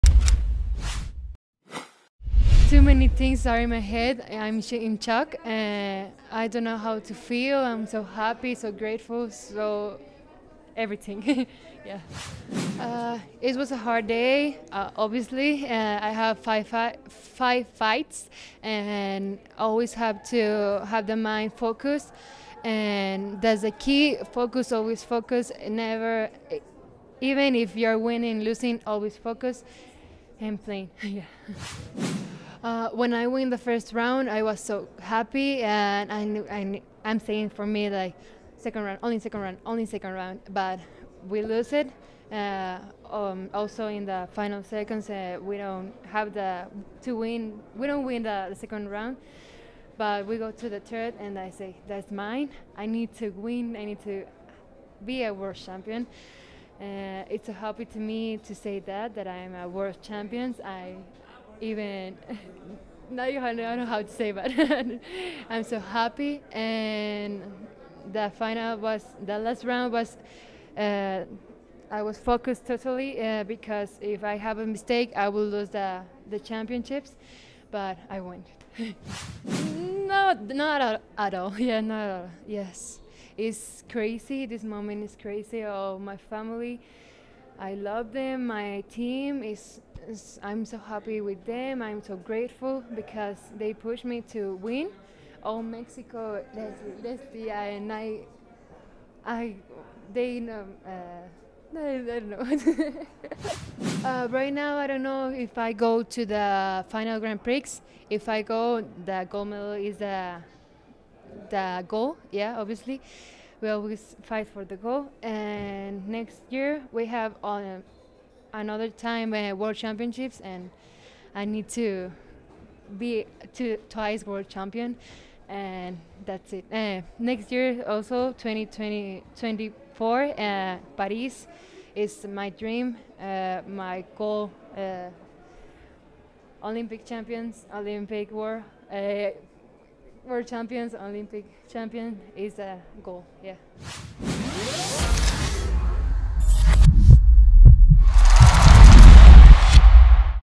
LESLIE SOLTERO POST FIGHTS INTERVIEW GUADALARAJA 2022 WT CHAMPIONSHIPS
LESLIE SOLTERO FROM MEXICO SPEAKS AFTER HER FIGHT AGAINST ALEKSANDRA PERISIC (SERBIA) IN THE FINALS OF THE GUADALARAJA 2022 WT CHAMPIONSHIPS